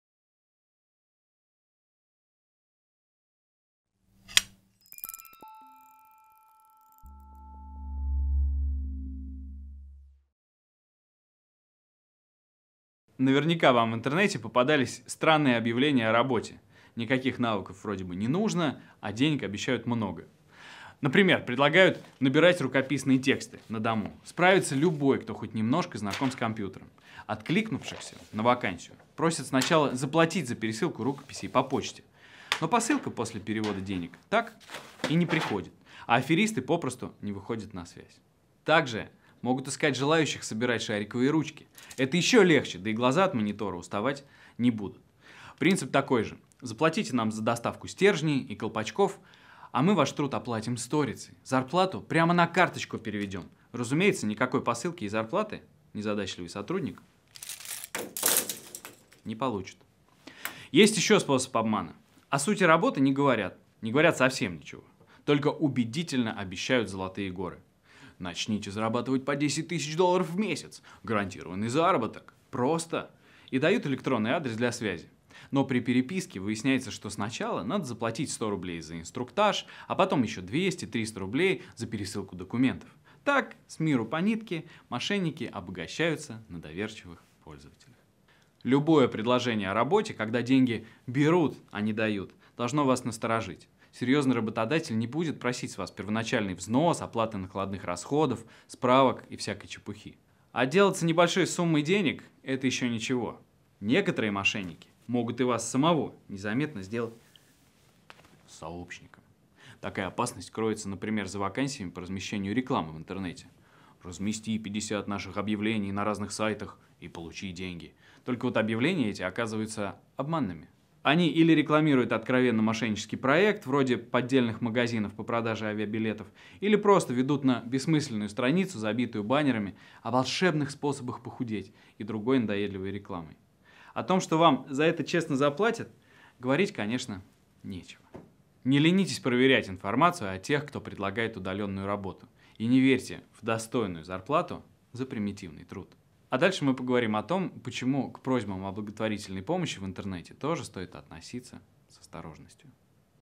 Безопасность в интернете - 18 урок. Безопасные онлайн-платежи. Работа, которой нет
Автор: Наталья Куканова, Надежда Кияткина, Александра Несанова

Наверняка вам в интернете попадались странные объявления о работе — никаких навыков как будто не нужно, а денег обещают много.

Предлагают, например, набирать рукописные тексты на дому — справится любой, кто хоть немного знаком с компьютером.

Откликнувшихся на вакансию сначала просят заплатить за пересылку рукописей по почте. Но посылка после перевода денег так и не приходит, а аферисты попросту не выходят на связь.

Также могут искать желающих собирать шариковые ручки — это ещё легче, да и глаза от монитора уставать не будут.

Принцип такой же —  заплатите нам за доставку стержней и колпачков, а мы потом ваш труд оплатим сторицей, зарплату прямо на карточку переведем. Разумеется, никакой посылки и зарплаты незадачливый сотрудник не получит.

Есть и другой способ обмана: о сути работы не говорят совсем ничего, только убедительно обещают золотые горы. «Начните зарабатывать по десять тысяч долларов в месяц! Гарантированный заработок – просто!». Но при переписке выясняется, что сначала надо заплатить 100 рублей за инструктаж, а потом еще — 200-300 рублей за пересылку документов. Так с миру по нитке мошенники и обогащаются на доверчивых пользователях.

Любое предложение о работе, когда деньги берут, а не дают, должно вас насторожить. Серьёзный работодатель не будет просить с вас первоначальный взнос, оплаты накладных расходов, справок и прочей чепухи.

Отделаться небольшой суммой — это ещё ничего. Некоторые мошенники могут и вас самого незаметно сделать сообщником.

Такая опасность кроется, например, за вакансиями по размещению рекламы в интернете. Размести 50 наших объявлений на разных сайтах и получи деньги. Только вот объявления эти оказываются обманными. Они или рекламируют откровенно мошеннический проект (вроде поддельных магазинов по продаже авиабилетов), или просто ведут на бессмысленную страницу, забитую баннерами о волшебных способах похудеть и другой надоедливой рекламой. О том, что за это вам честно заплатят, и говорить нечего.

Не ленитесь проверять информацию о тех, кто предлагает удалённую работу, и не верьте в достойную зарплату за примитивный труд.

А дальше мы поговорим о том, почему к просьбам о благотворительной помощи в интернете тоже надо относиться с осторожностью.